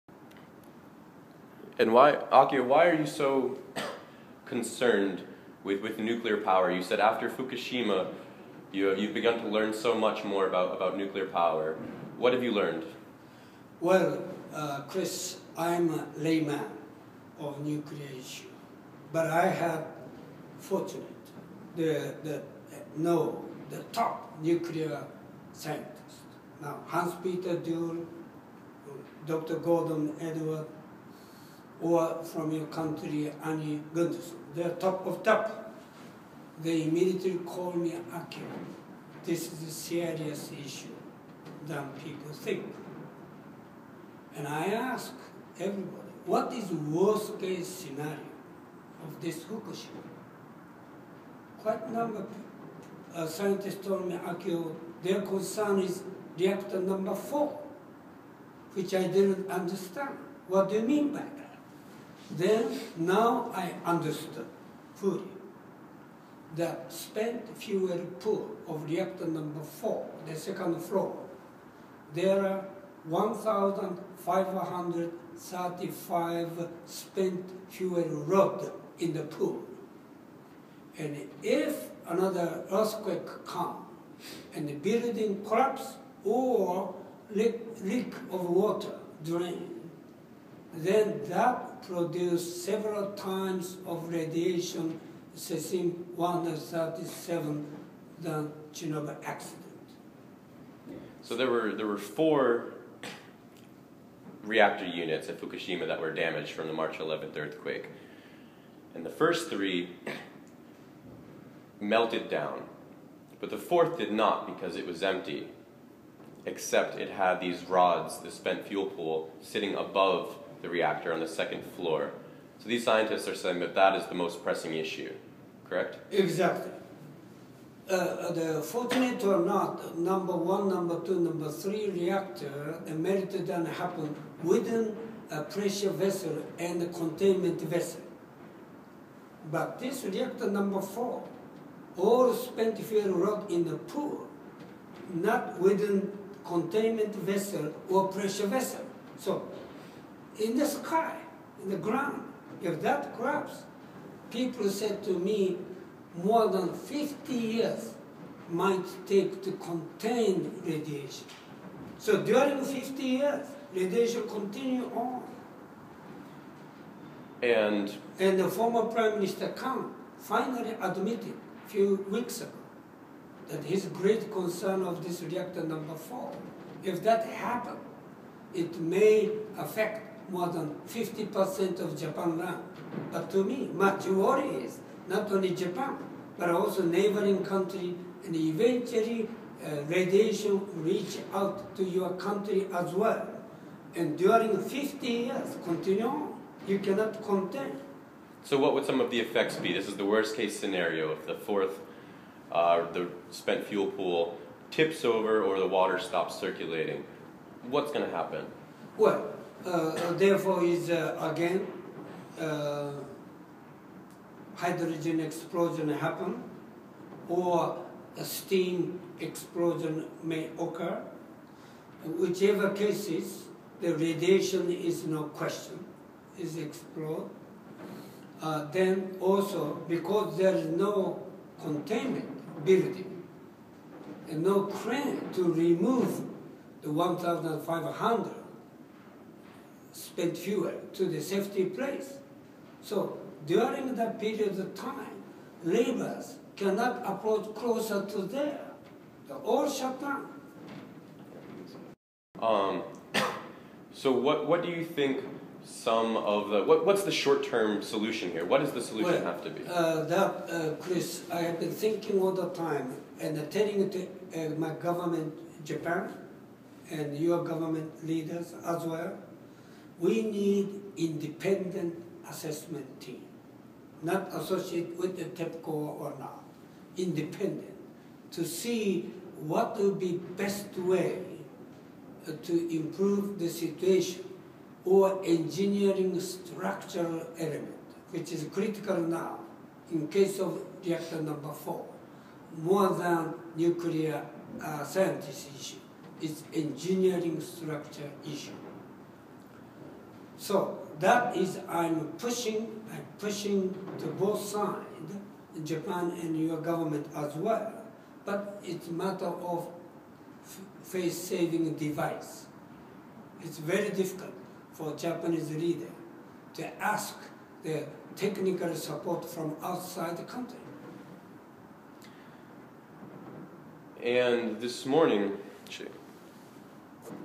Fukushima Reactor 4
Akio Matsumura talks about nuclear power plants, spent fuel pools, and the trouble with Reactor 4 at Fukushima.